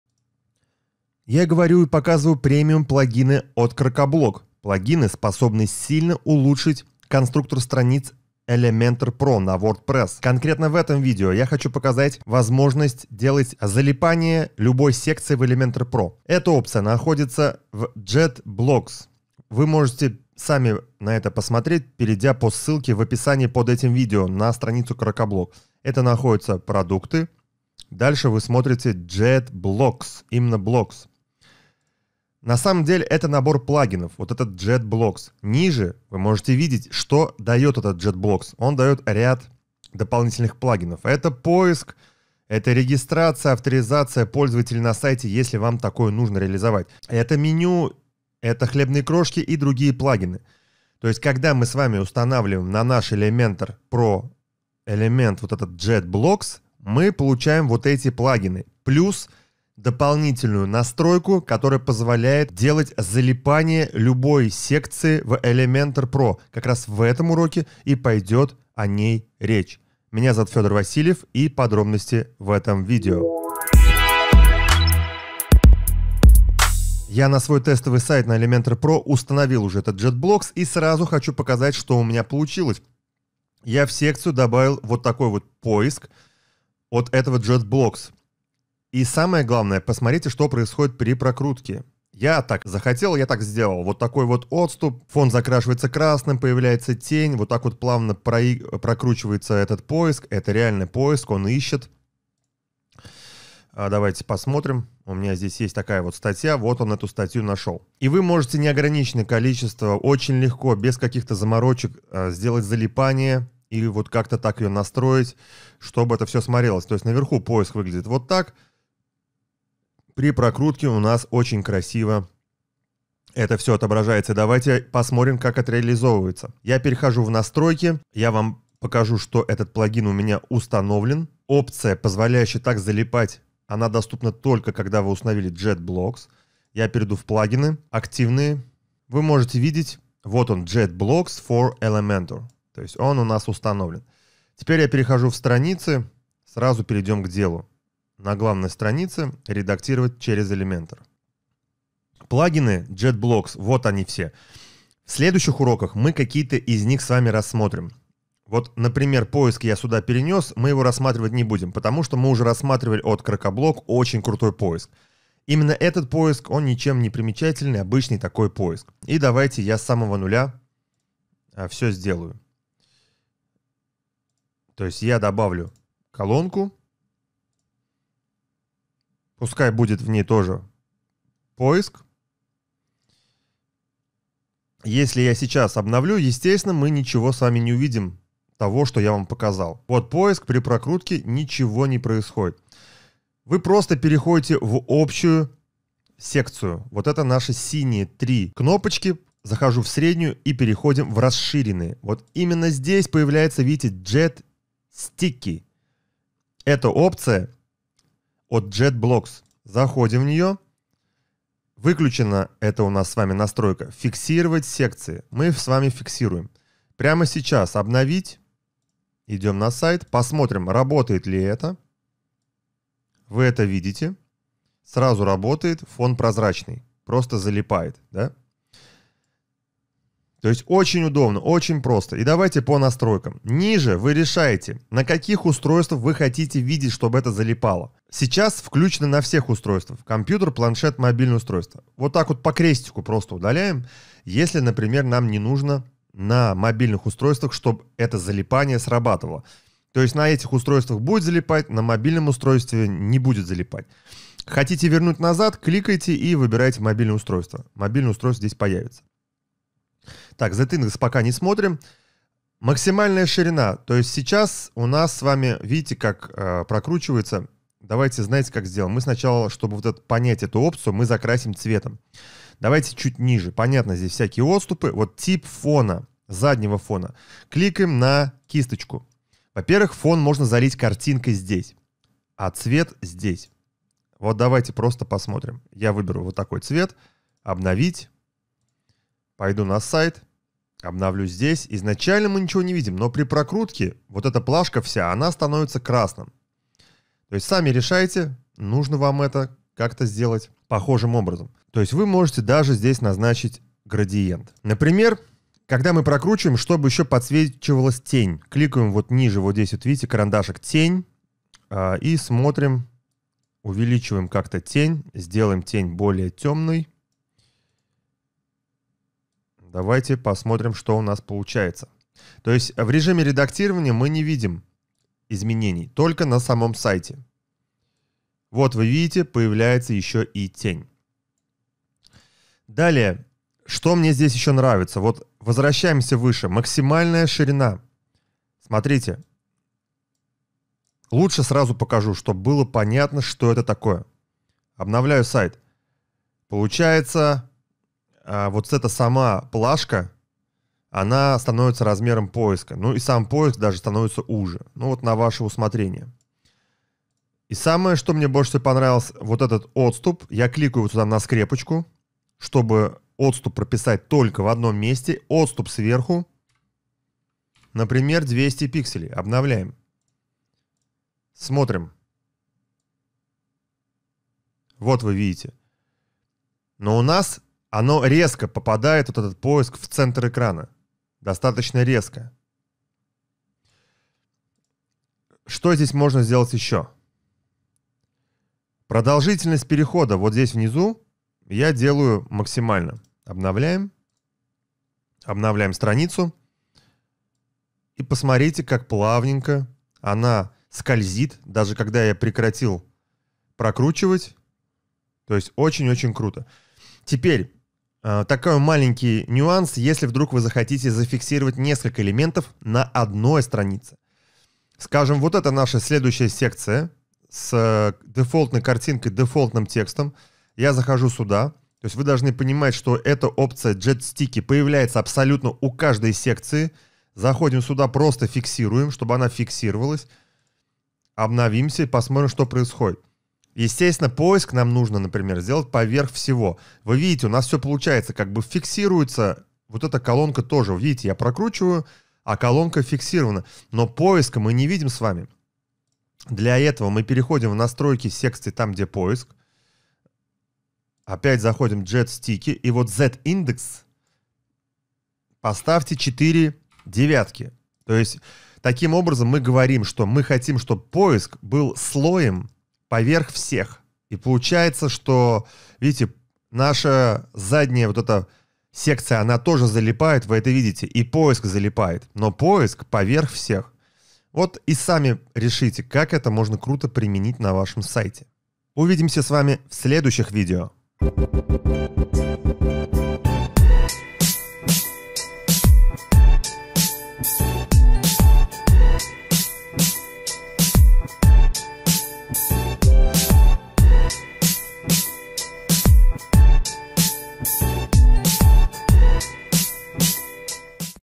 Crocoblock — JetBlocks, Jet Sticky (Липкая колонка) Elementor PRO, WordPress #16
В этом видео я покажу функцию от JetBlocks, которая позволяет зафиксировать любую строку Elementor при прокрутке страницы, а также добавить эффекты:  задать тень, задать цвет, отступ и скорость анимации прокрутки.